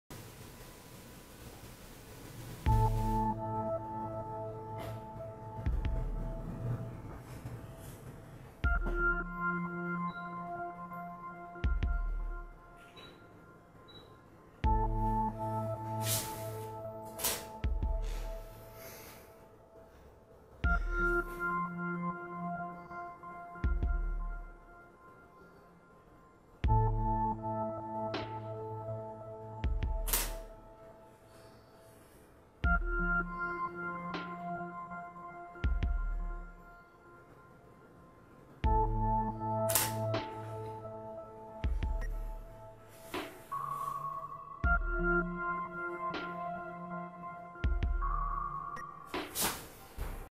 PK Tether interval shooting
Still life photography session using Pentax K-5 IIs tethered with PK_Tether by Tomasz Kos. Interval shooting helps me having both my hands free while repositioning the subject.